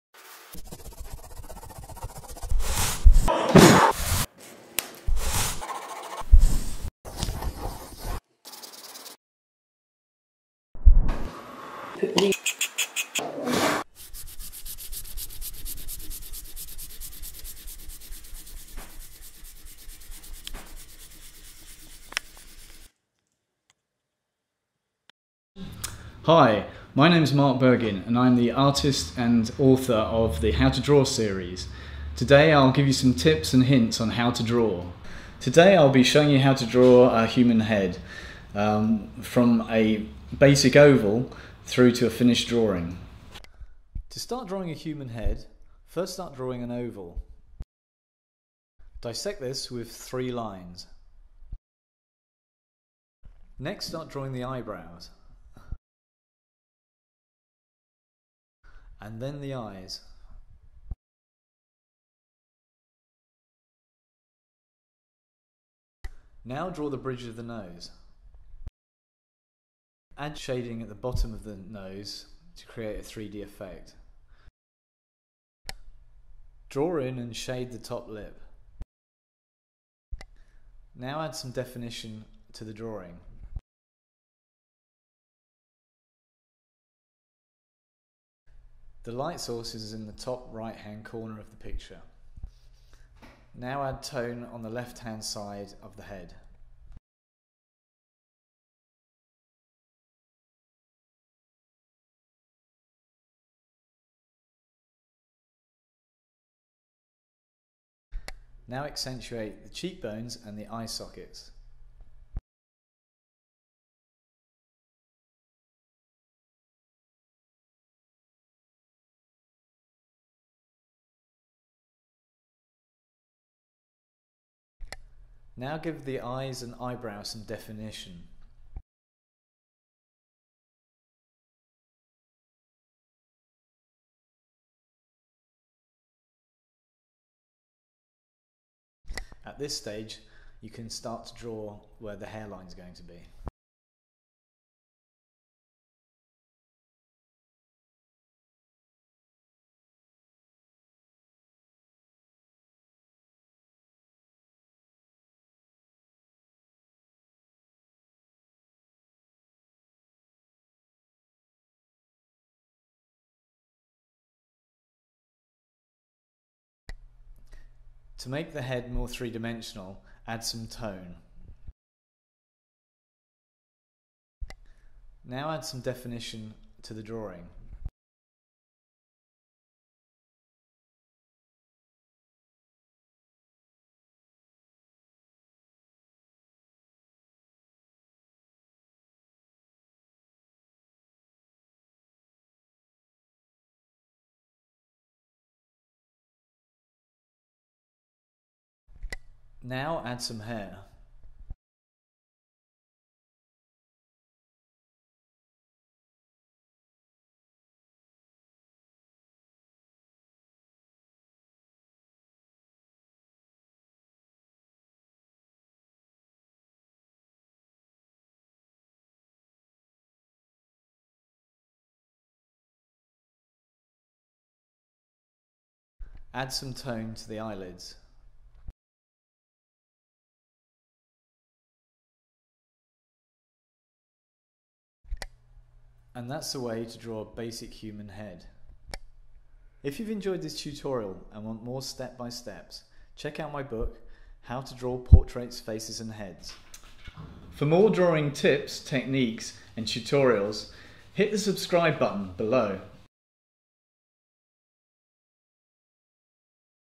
How to Draw a Human Face step-by-step by Mark Bergin
Illustrator Mark Bergin guides you through step-by-step instructions on how to draw a human face.